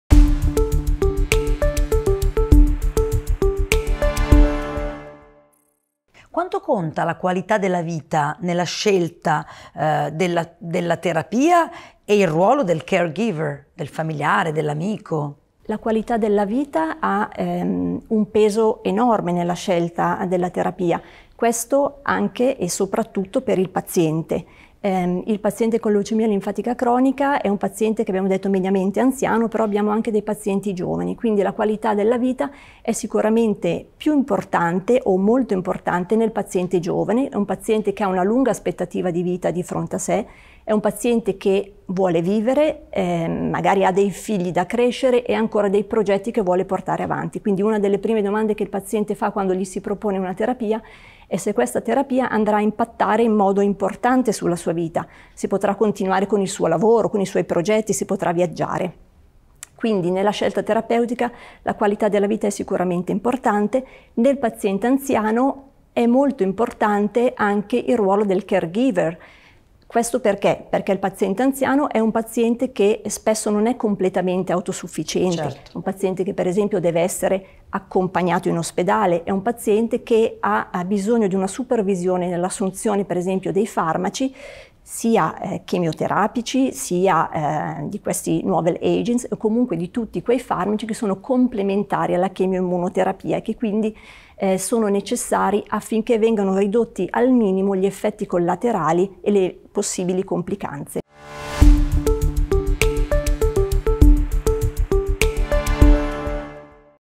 CLL: Quanto pesa la qualità della vita nella scelta della terapia? E il ruolo del caregiver?
La qualità della vita ha un peso enorme nella scelta della terapia, soprattutto per il paziente. Nel paziente anziano, che può non essere completamente autosufficiente, è altrettanto importante il ruolo del caregiver, che deve supervisionarlo nell’assunzione dei farmaci e accompagnarlo alle visite.
Risponde la Dr.ssa Marina Motta.